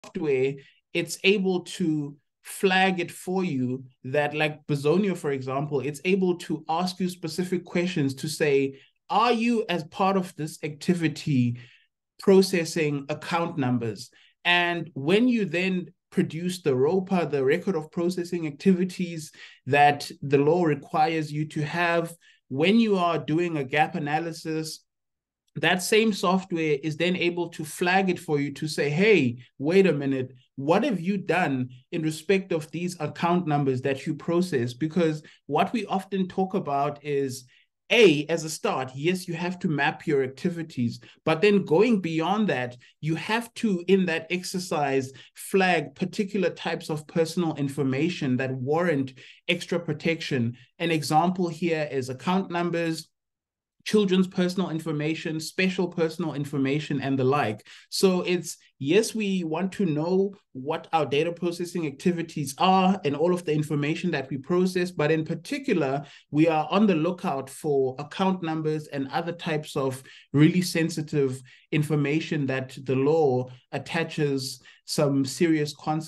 Michalsons | Managing risk by protecting account numbers
In one of our weekly webinar sessions, Sicelo Kula discusses the necessity of mapping activities to understand when and why an organisation handles account numbers.

To participate in these talks, sign up for one of Michalsons' various programmes.